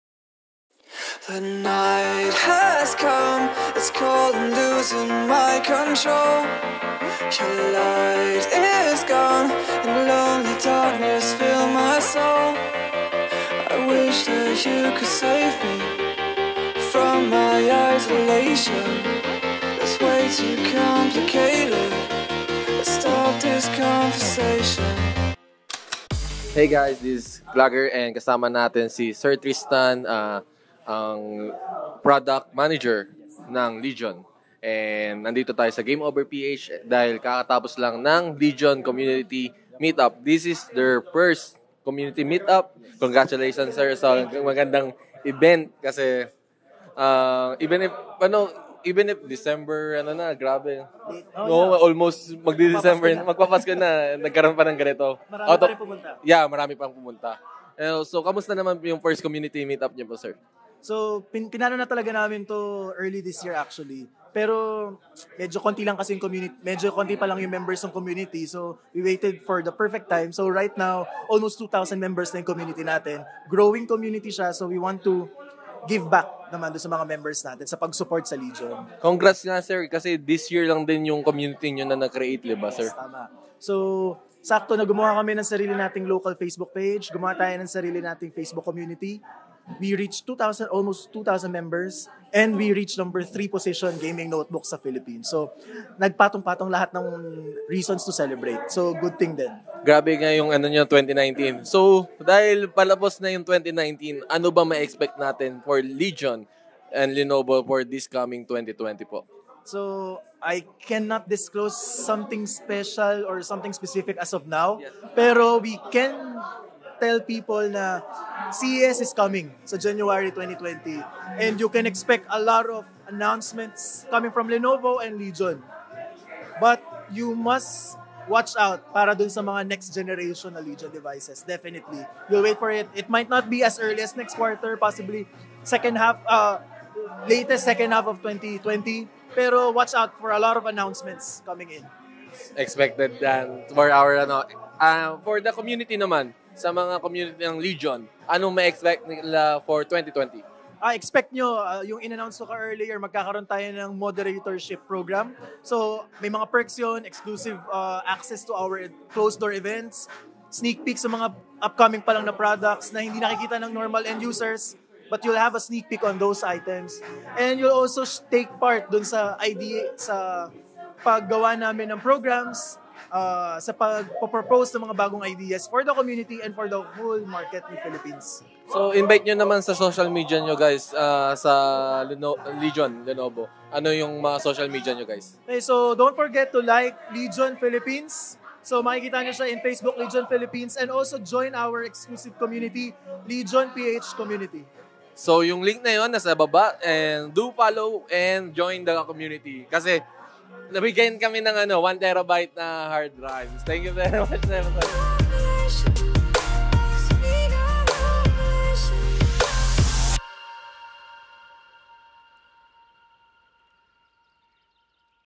First Legion Philippines Community Gathering
First Legion Philippines Community Gathering happened on December 21, 2019 @ Game Over Philippines

Buy your things at Lazada!

Are you looking for a Best VPN? Try VyprVPN!